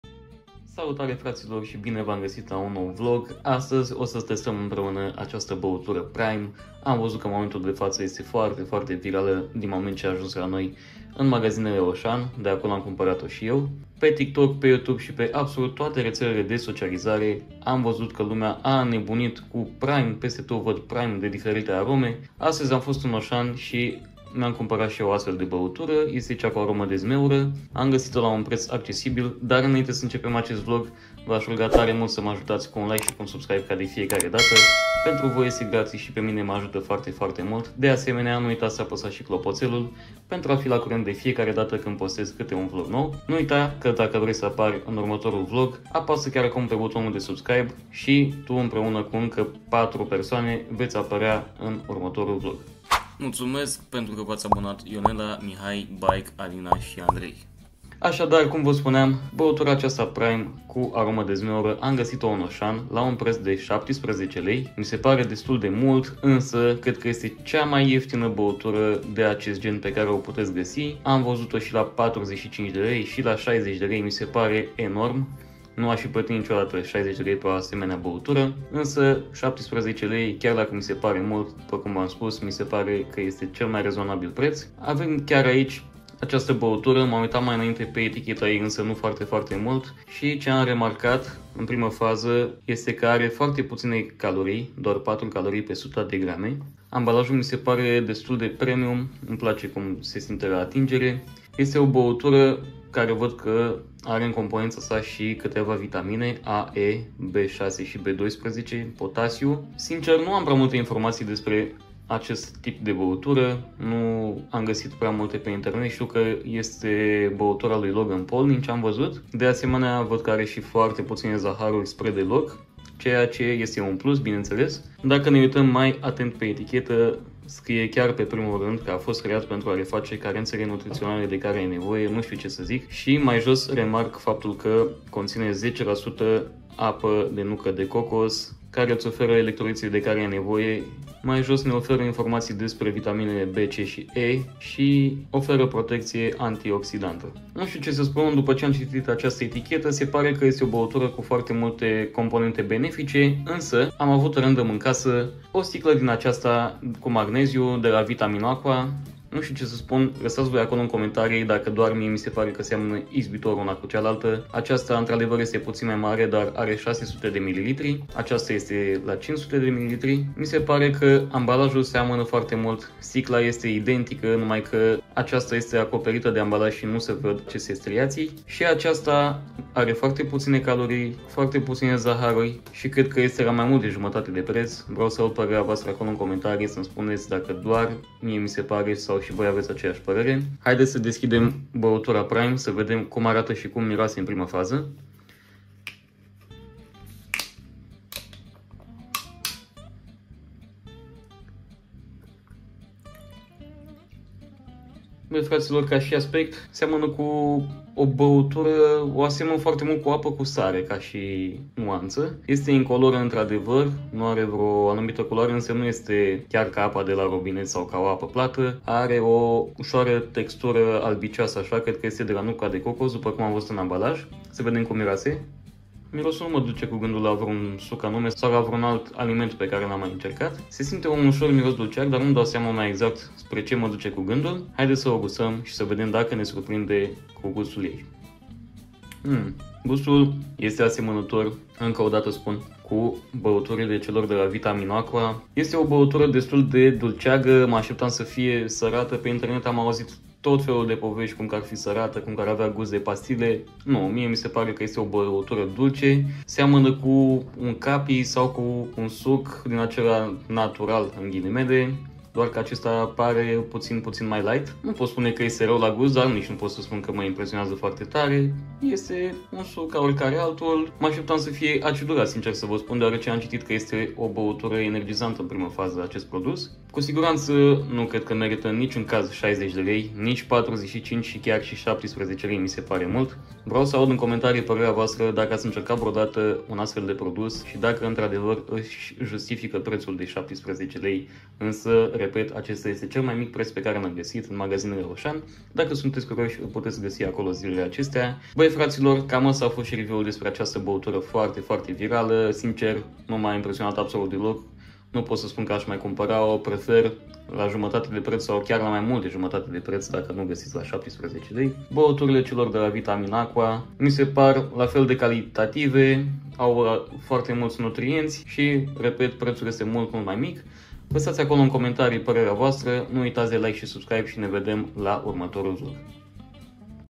AM TESTAT BAUTURA PRIME *Merita 60 de lei?!* 🤔
Bai fratilor, datorita hype-ului pe care l-am regasit in ultima perioada pe toate retelele de socializare cu aceasta bautura "PRIME" am decis sa o cumpar, sa o testez si sa va spun si voua cateva cuvinte despre ea.Lasa in comentarii daca ai incercat-o vreodata, vreau sa aflu si parerea ta despre ea.Pofta buna!